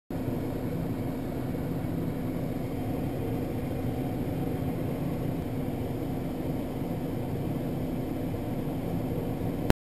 room view
a view from the window of the room we stayed in at the elk view inn in the big horn mtns wyoming